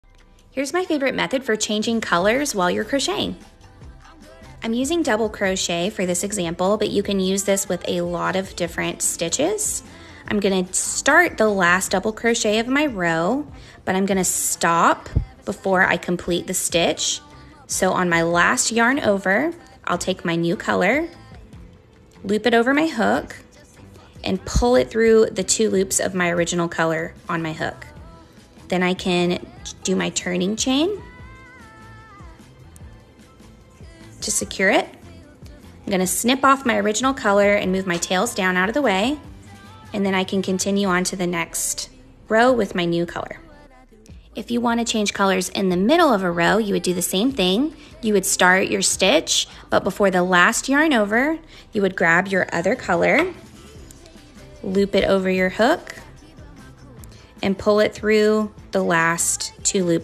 Easy crochet tutorial! How to change yarn color crochettips crochettutorial 🧶👏🏻
How do you change crochet colors??

There are several ways to change yarn color but this is my favorite crochet method!

Yarn: @lionbrandyarn Lazy Days